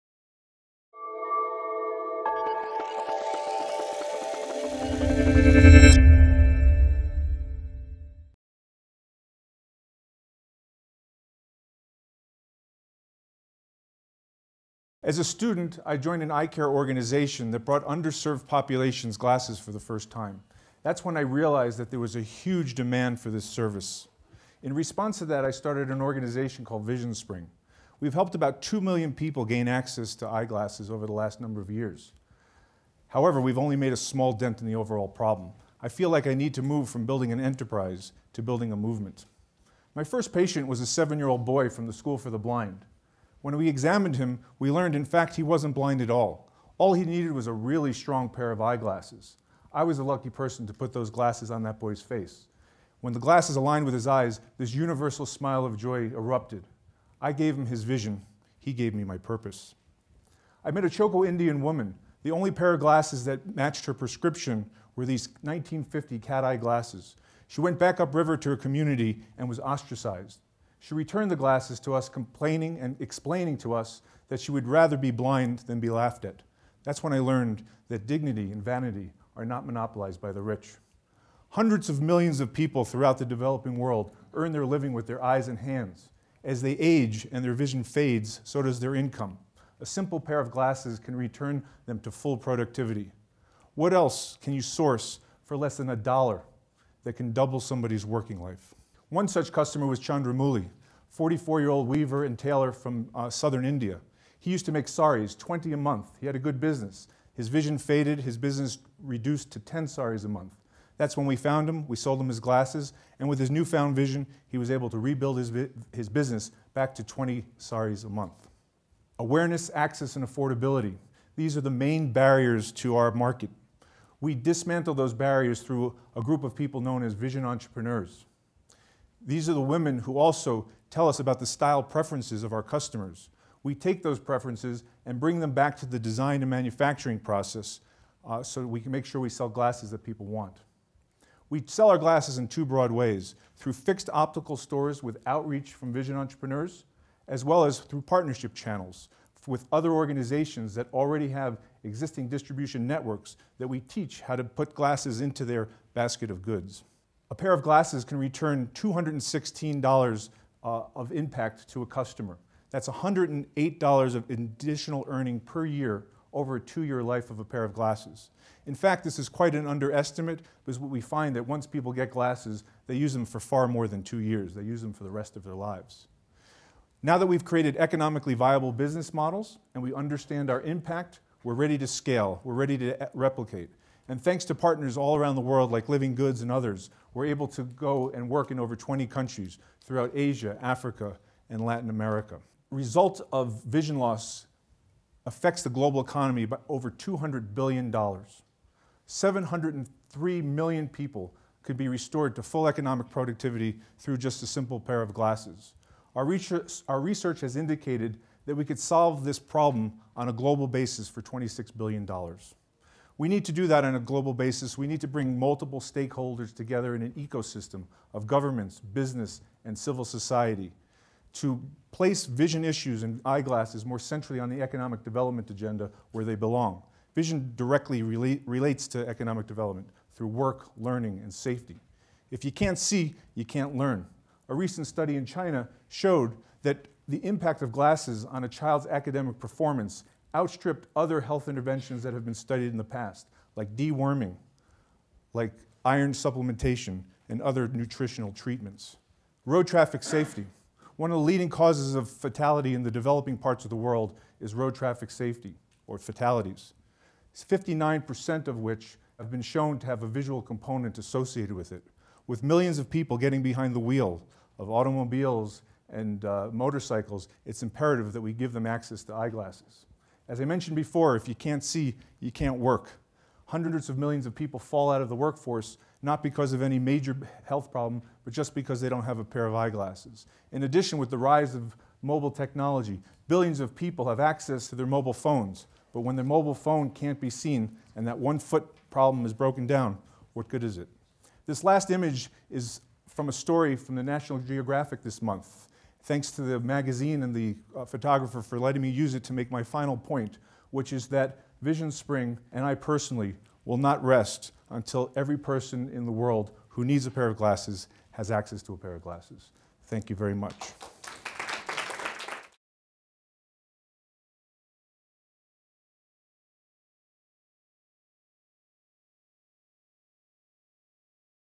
IdeasLab 2014 - Jordan Kassalow - Why Does Vision Matter?
Over 703 million people could get their sight back with a simple pair of glasses. Jordon Kassalow from VisionSpring USA discusses the economic impact of glasses, and calls for global partners to make vision a priority.